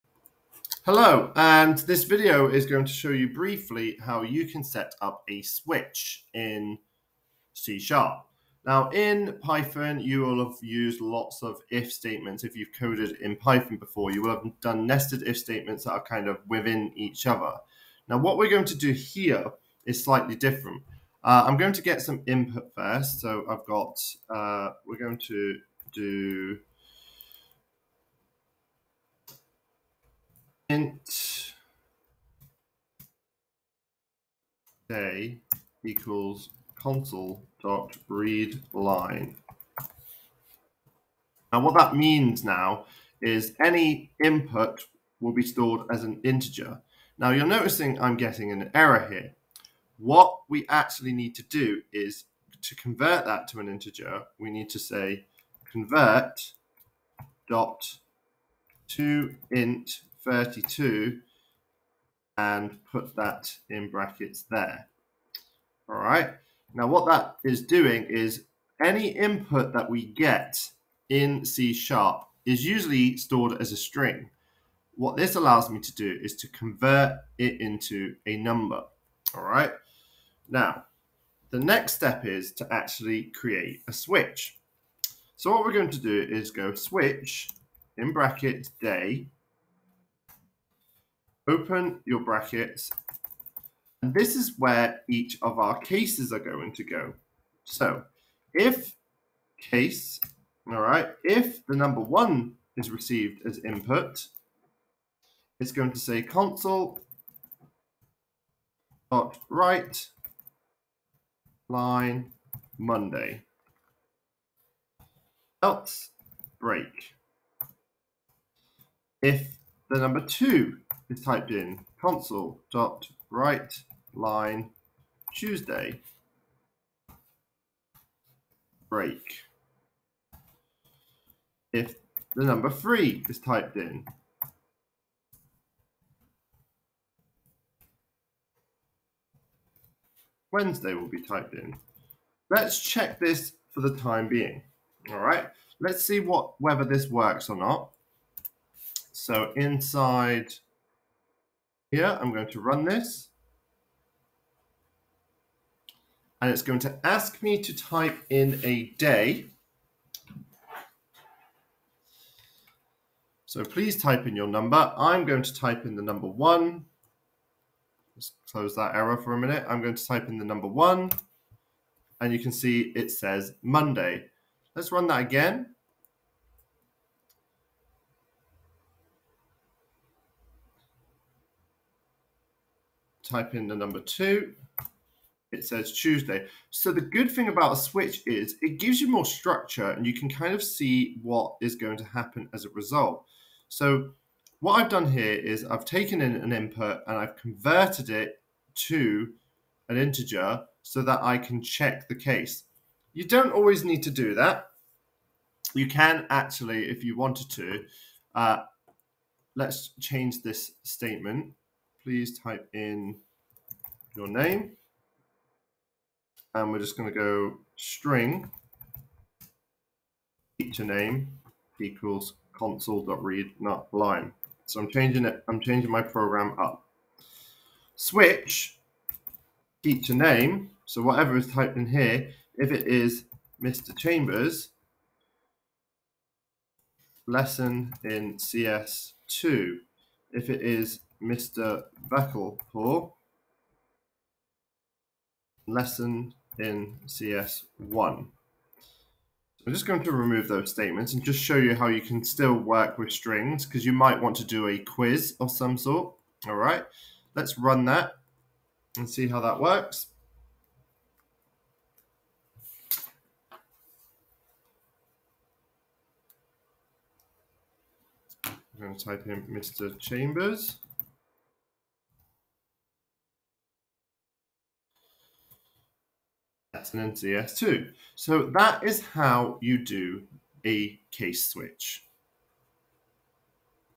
Lesson 7 - C# Switch Case
This video introduces you how to do a Case Switch in C# and shows you how you can include multiple inputs to come up with an answer. This is perfect for a quiz in c sharpe